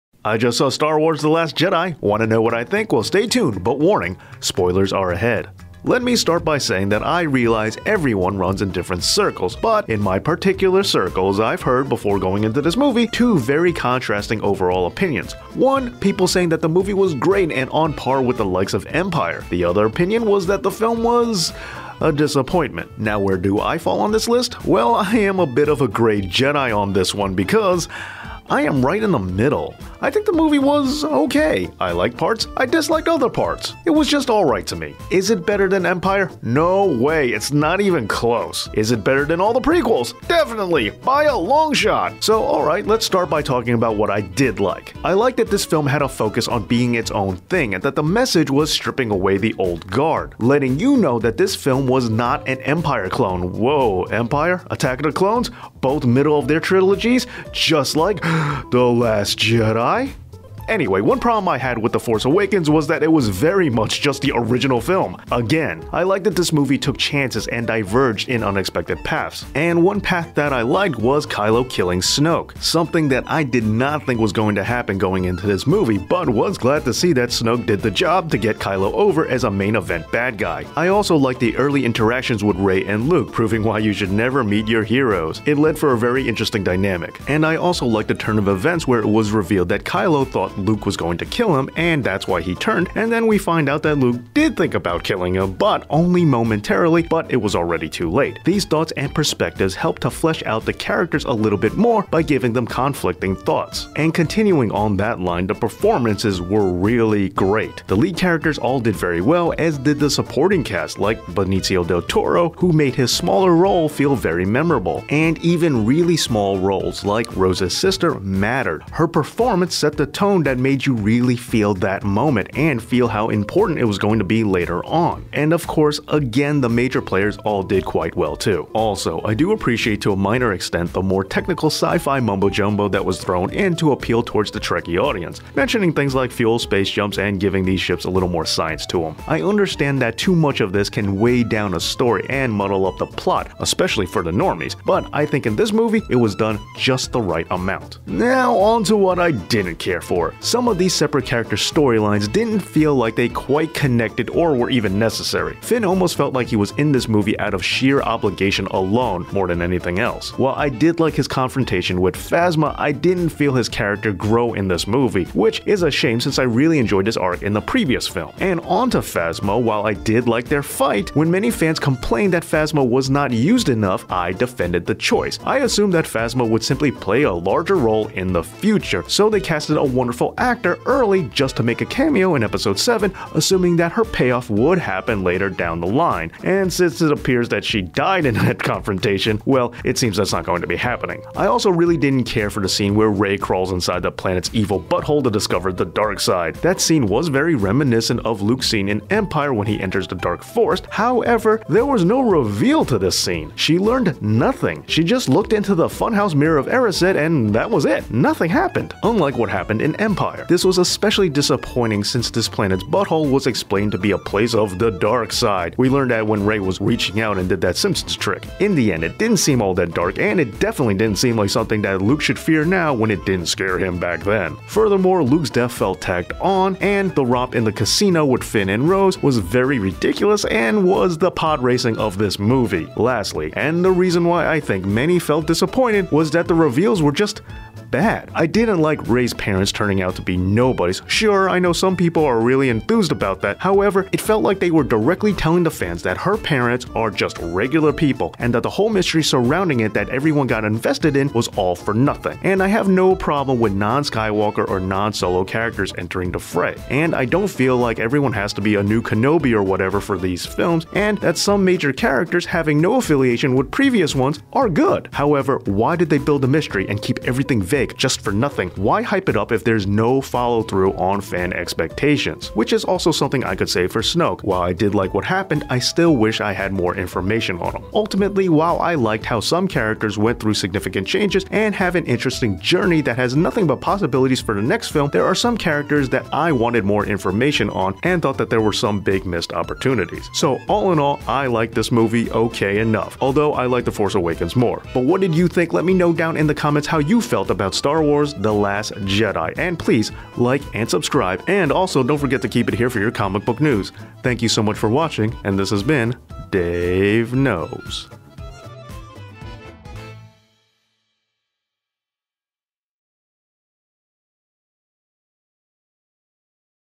STAR WARS: THE LAST JEDI Review!
DAVE KNOWS that the Last Jedi is as anticipated as any Star Wars film is. However, is it worth the hype? With fans divided on the film, I am throwing my review in to talk about how I felt the movie stacked up.